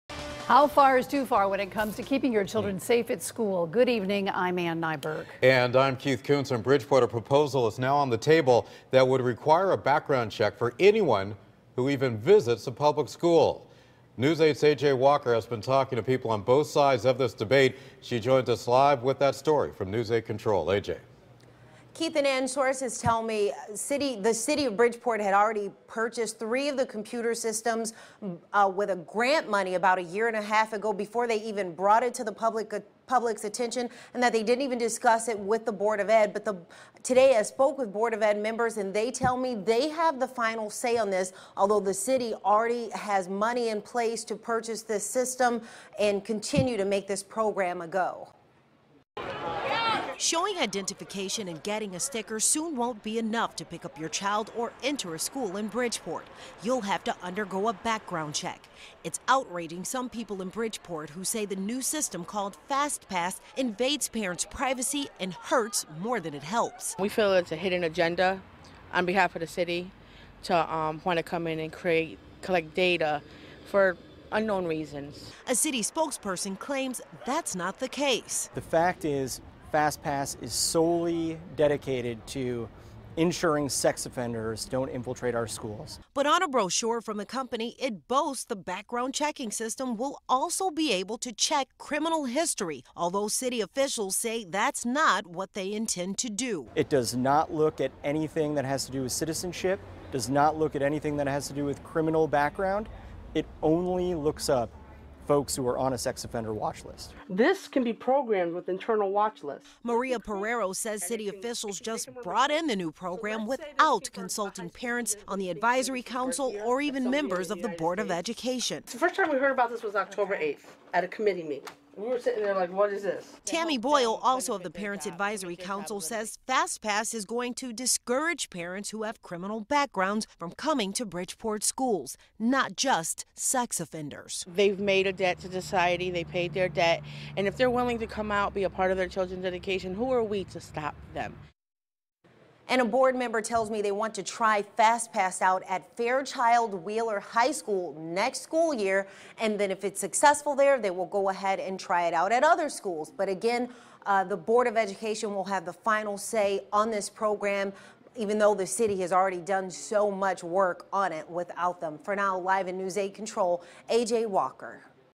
Bridgeport school background checks
The  city of Bridgeport plans to implement a new system that will make people visiting schools undergo a background check to get in the building.